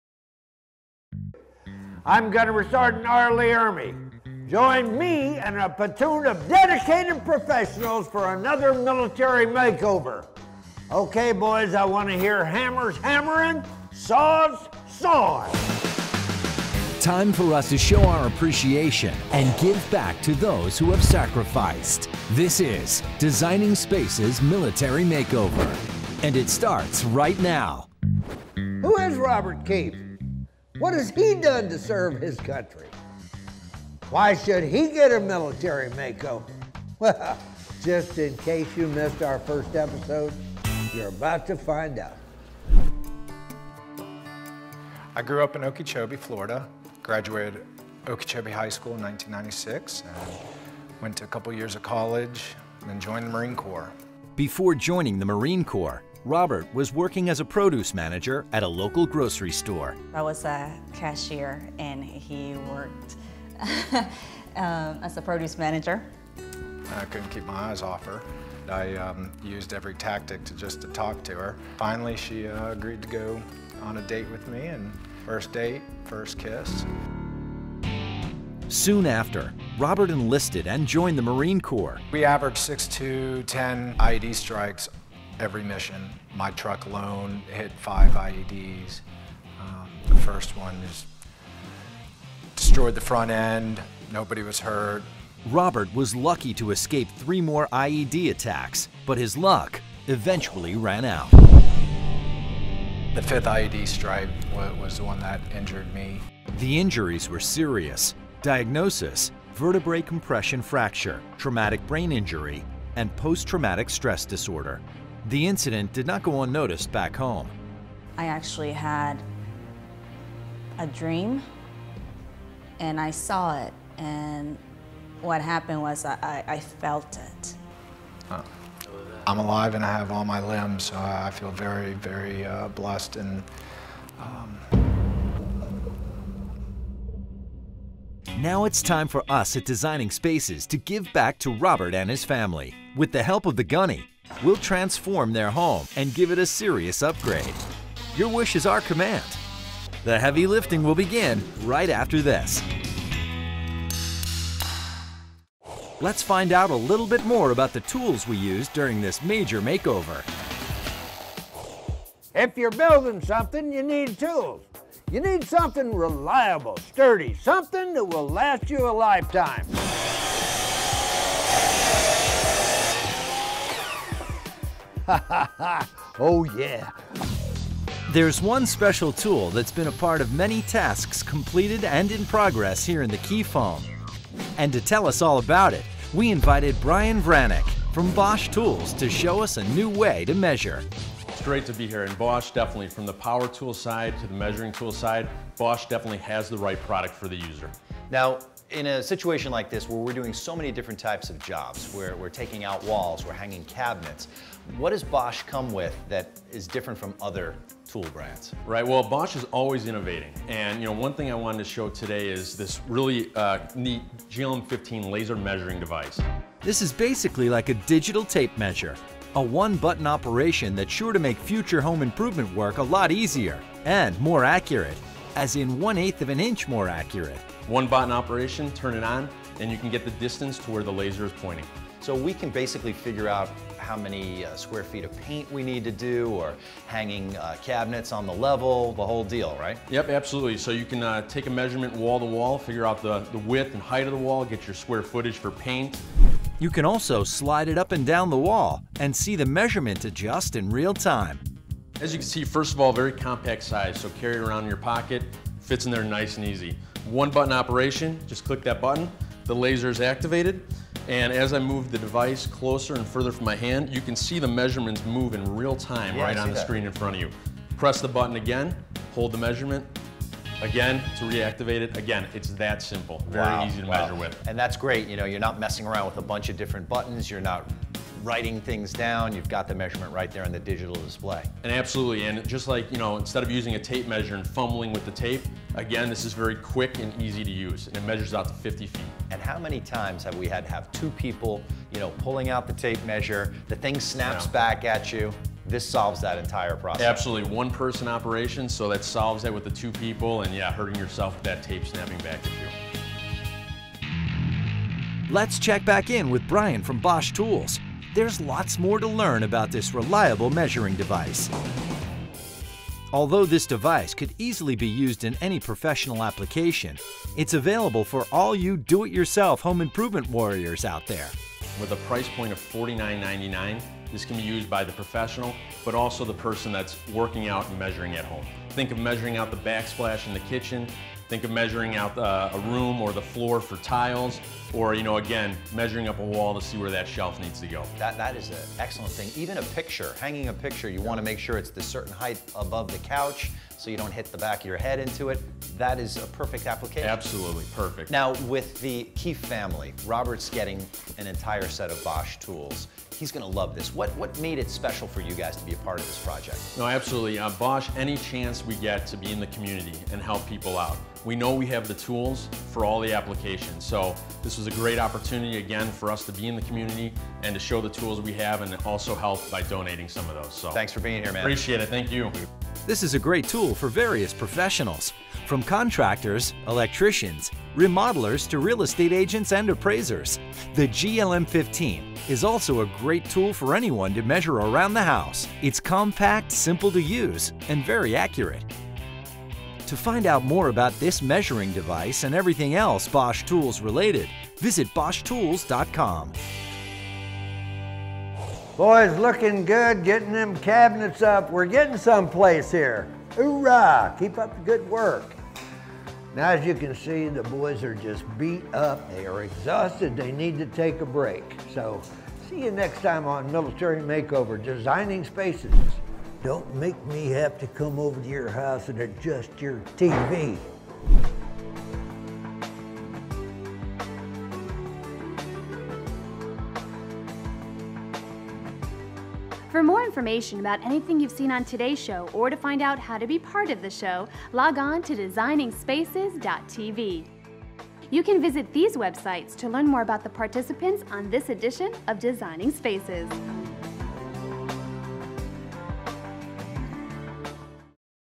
Measure Twice, Cut Once - Designing Spaces
Art Edmonds checks out the new BOSCH GLM 15 Laser Measure, a replacement for a tape measure that can be used in many different DIY and home improvement applications. Brian Vranek from Robert Bosch Tool Company explains how Bosch is always innovating, and the laser measure is one of their latest. This “digital tape measure” eliminates fumbling with awkward metal tapes on traditional tape measures. It can quickly become one of your most indispensable tools in your tool box. And for Robert Keefe, the BOSCH GLM 15, along with some nice Bosch power tools, are gifted to him by the good folks at Robert Bosch Tool Corp.

102379